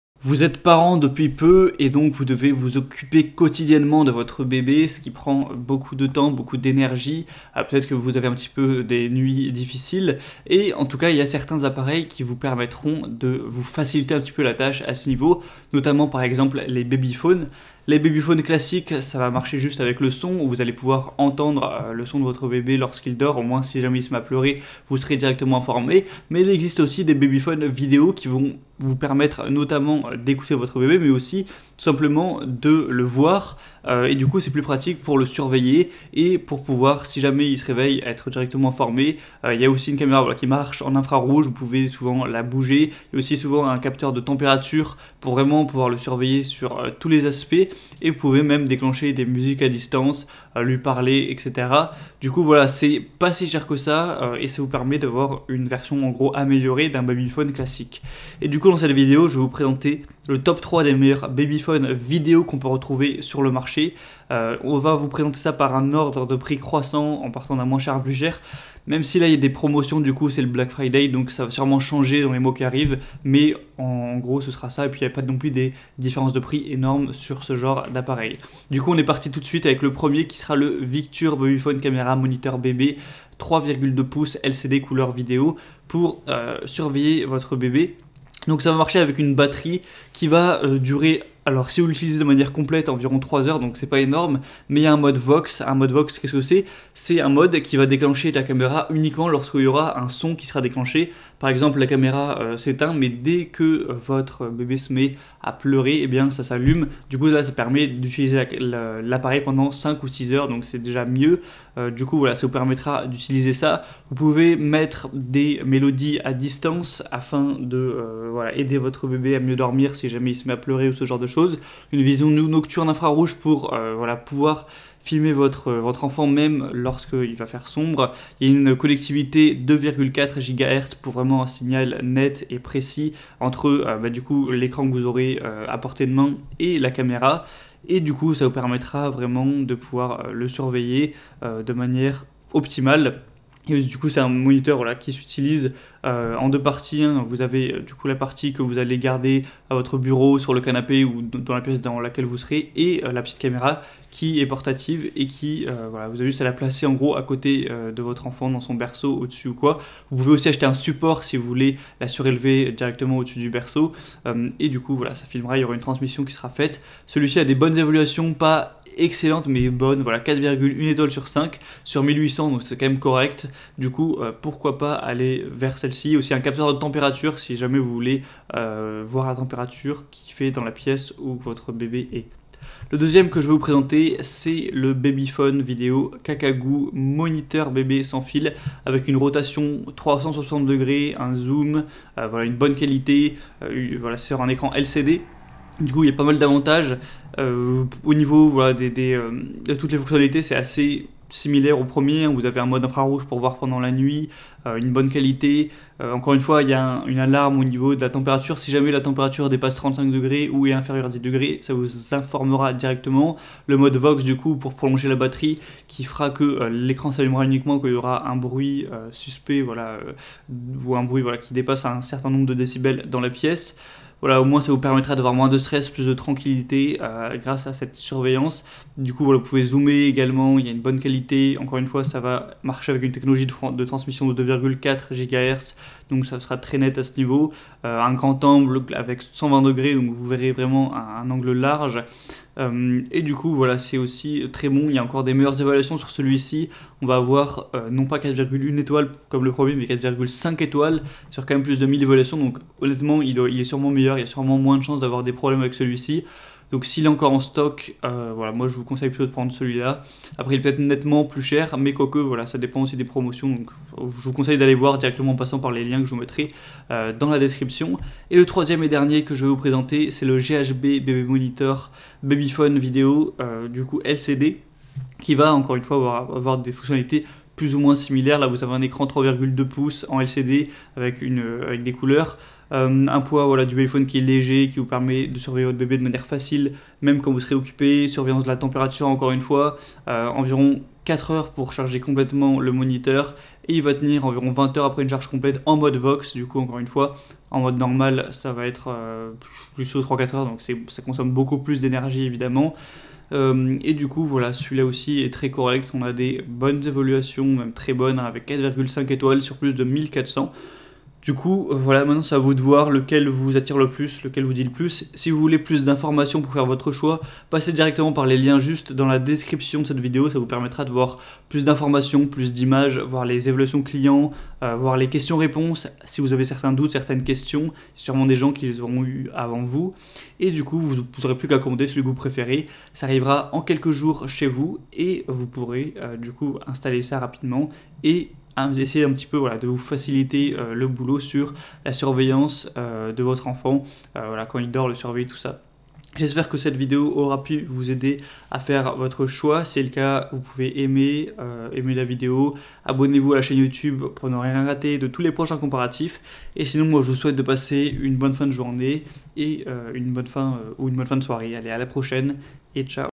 TOP 3 : Meilleur Babyphone Vidéo 2020
⭐Victure Babyphone Caméra Moniteur bébé
⭐Babyphone Video CACAGOO Moniteur Bébé sans Fil avec Rotation 360°
⭐GHB Bébé Moniteur Babyphone Vidéo LCD Couleur Caméra Bébé

Pour surveiller votre bébé à distance dans les meilleures conditions, offrez vous un babyphone vidéo !

Dans cette vidéo je vous présente le top 3 des meilleurs babyphone vidéo caméra pas cher et à commander chez soi de l'année 2020 ! Bien Chez Soi participe au programme Amazon Partenaires.